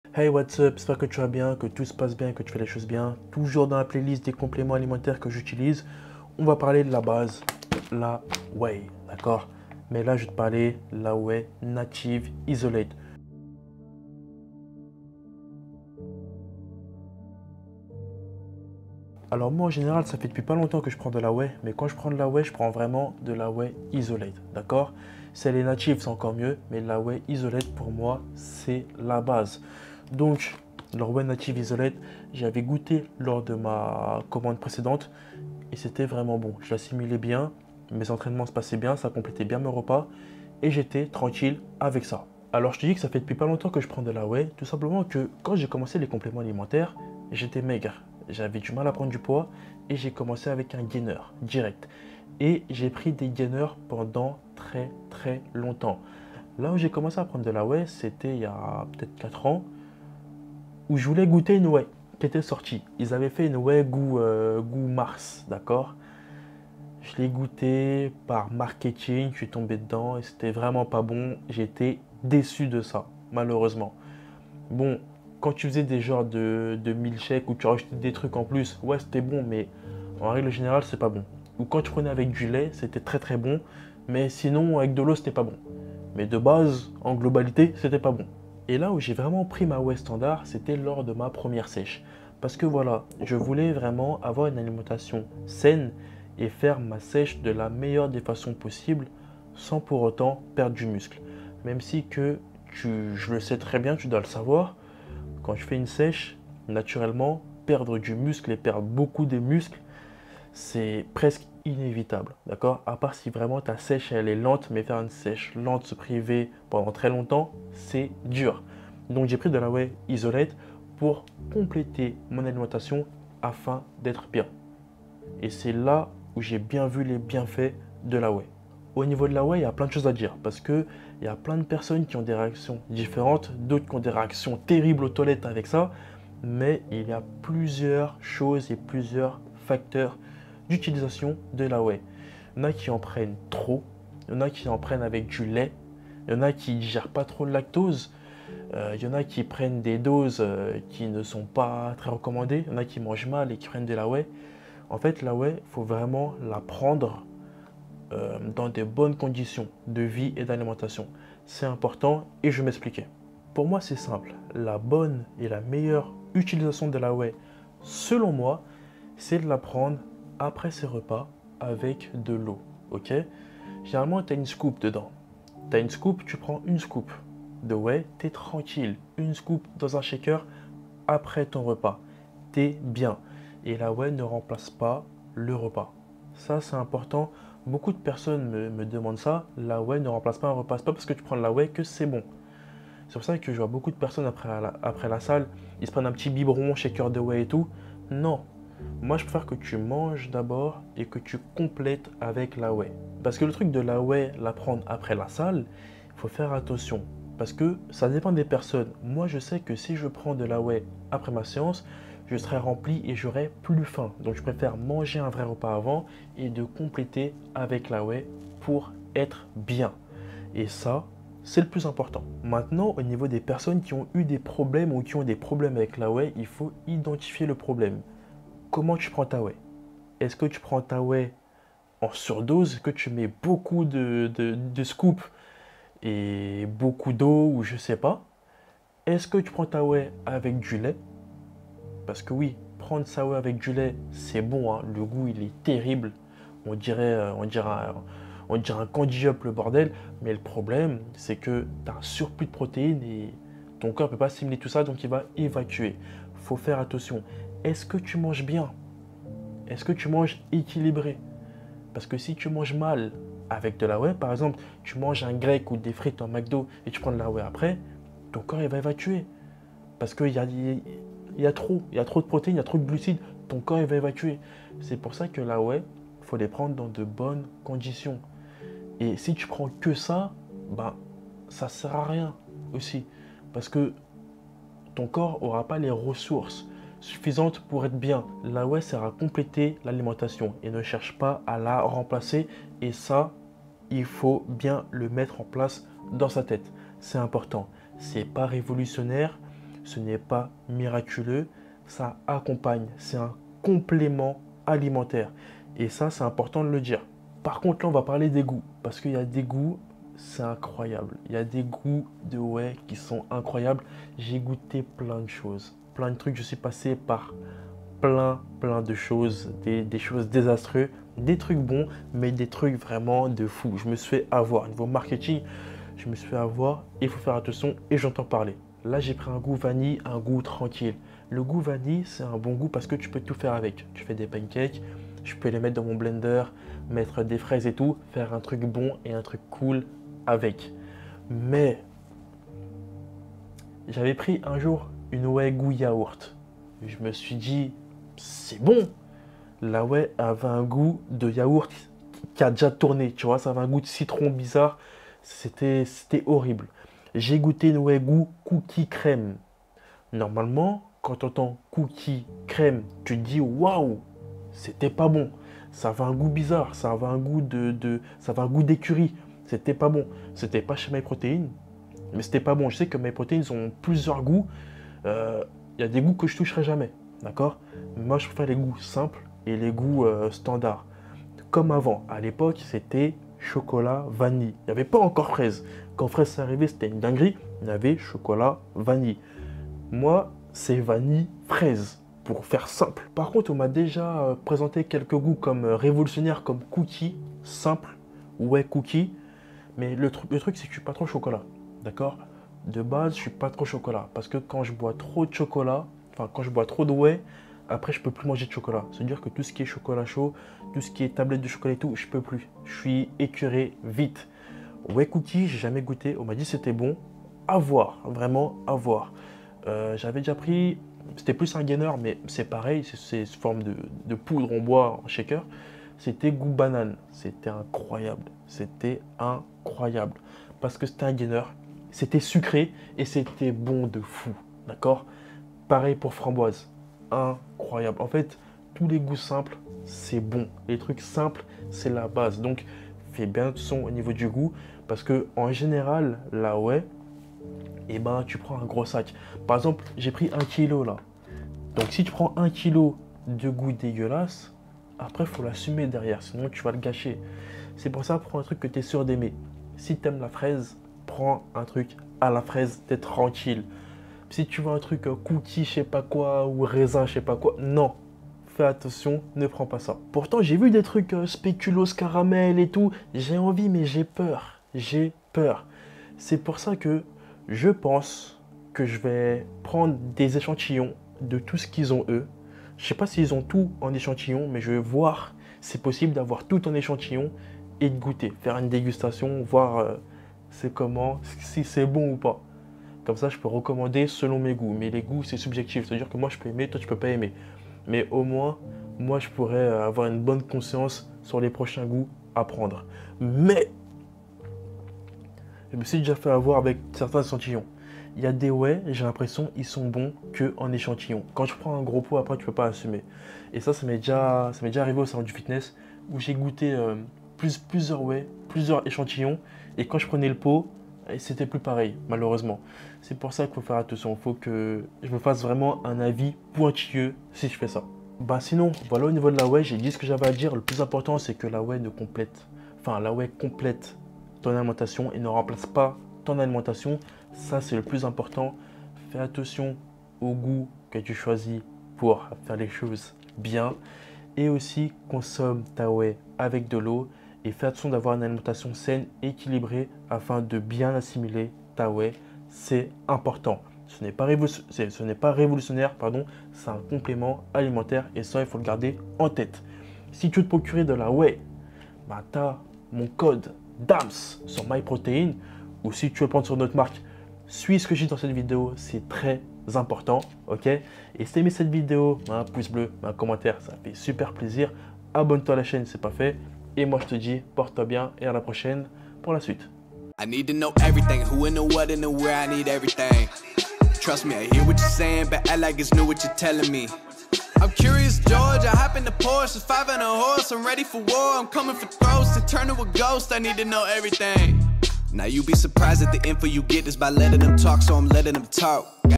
COMMENT JE PRENDS MA WHEY PROTEIN !?
Aujourdhui petite video sur ma prise de la whey c’est pareil pour les gainer !!